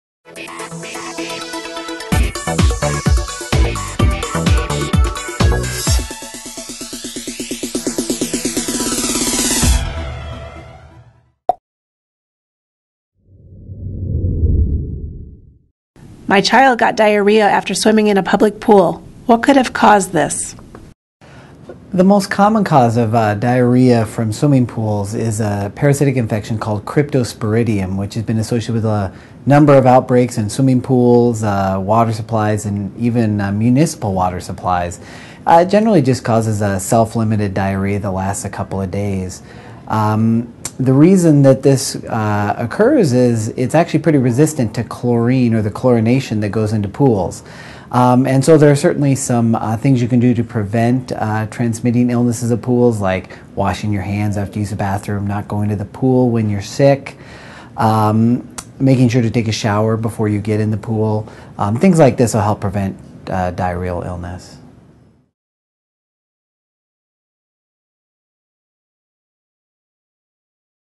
AskUNMC: Swimming & Diarrhea
My child got diarrhea after swimming in a public pool. What could have caused this?

Trevor VanSchooneveld, M.D.,
UNMC College of Medicine